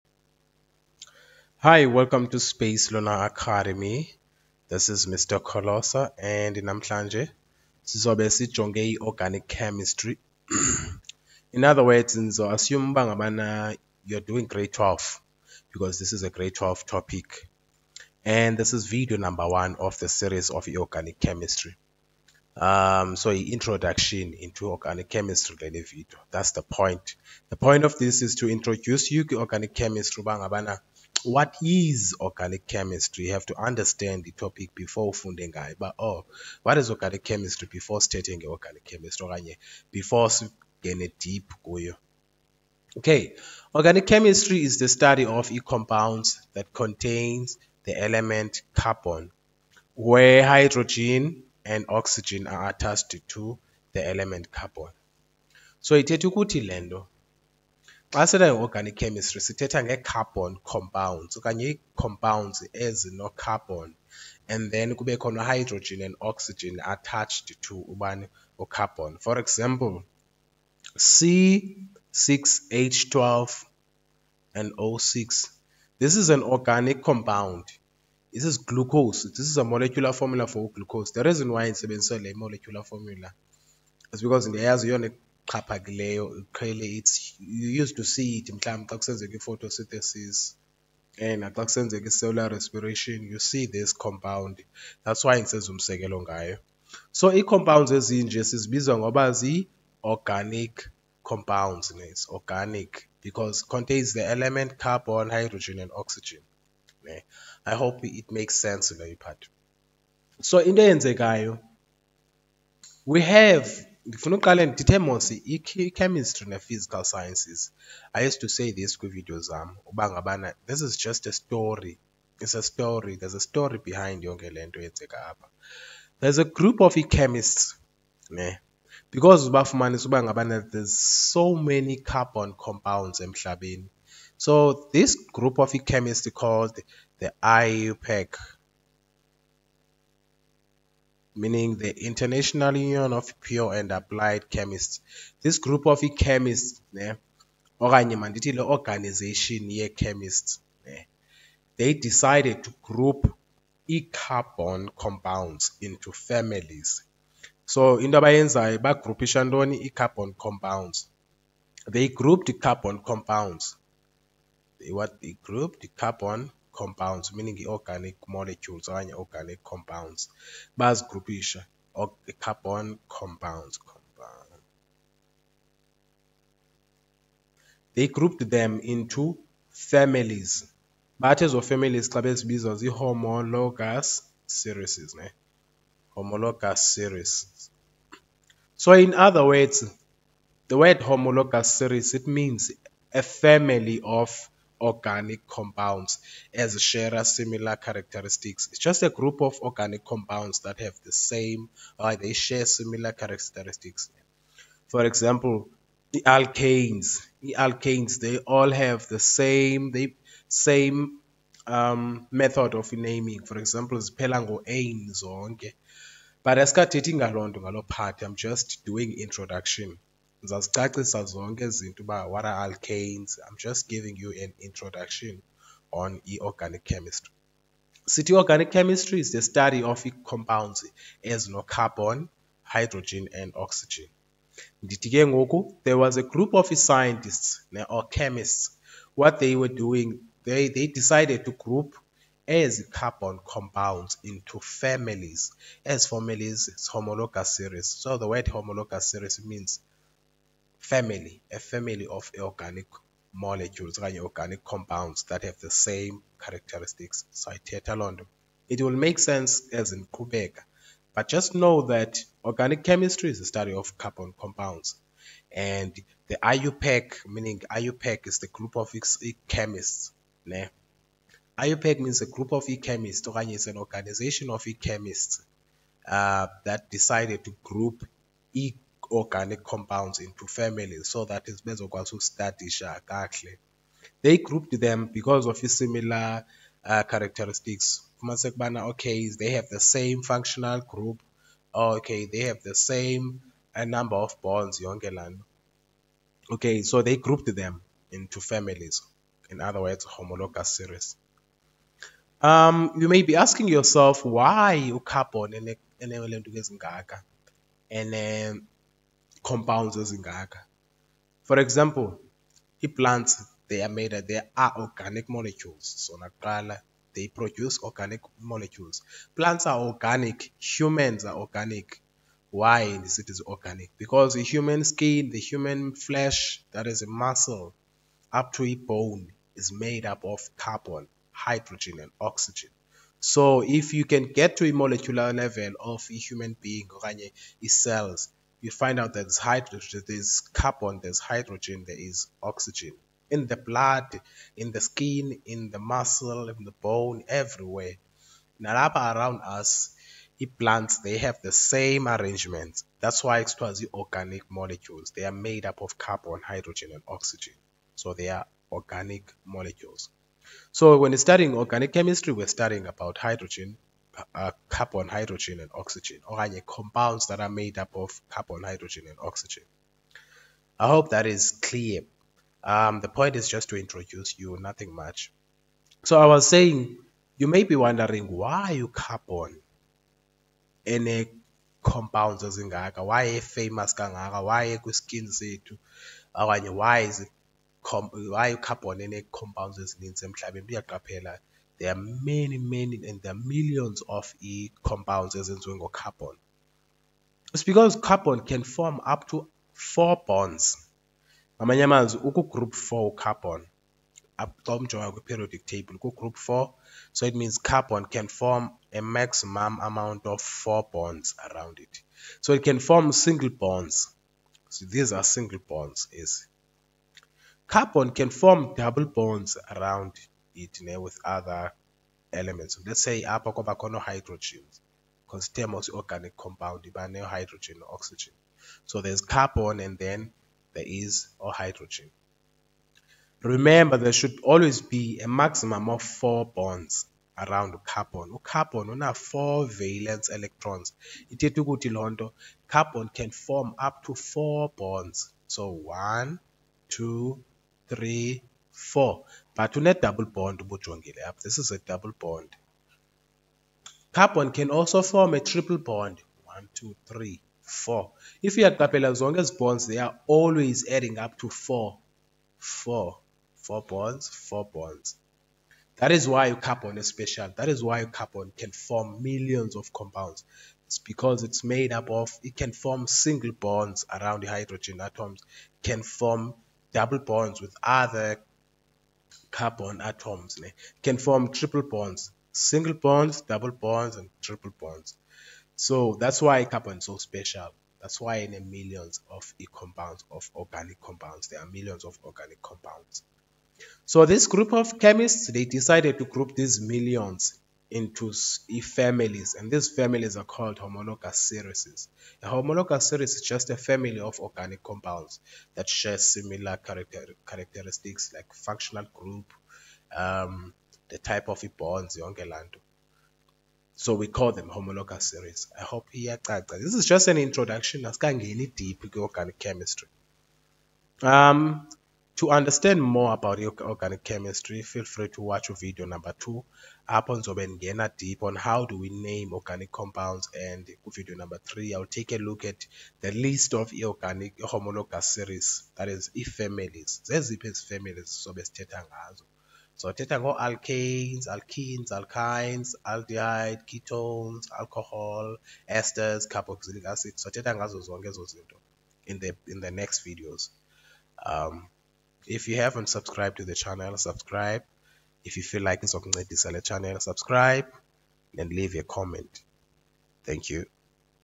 Grade 12 0rganic chemistry video:1 Part 1
In this video we will take a look at organic chemistry for grade 12 . We will explain the meaning of homologous series and IUPAC name of organic compounds.